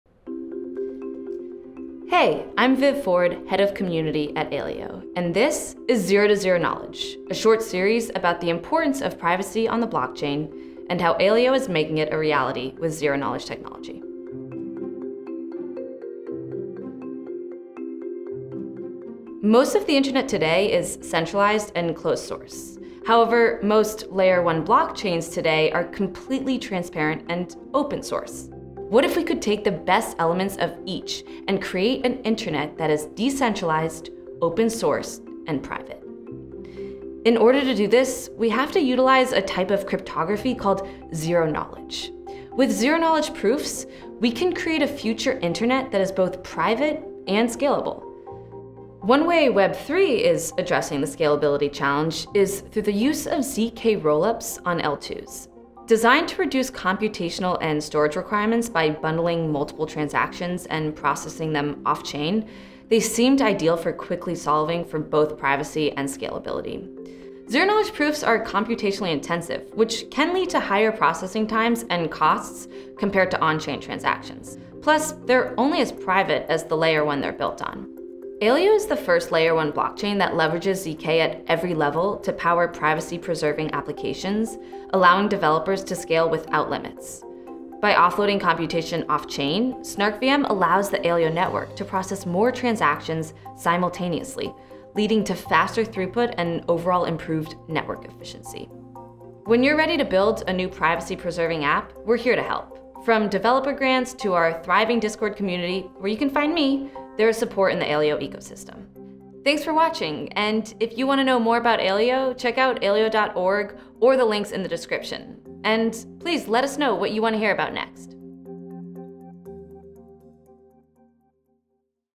Zero to Zero-Knowledge | Offchain Scalability with SnarkVM
The performance you get out of ZKPs heavily depends on the approach you take to generating them, as they're generally intensive on computation, you'd need a comprehensive solution to get both scalability and performance. Viviane Ford, Community Lead at the Aleo Network Foundation, explains why SnarkVM may just hit the sweet spot.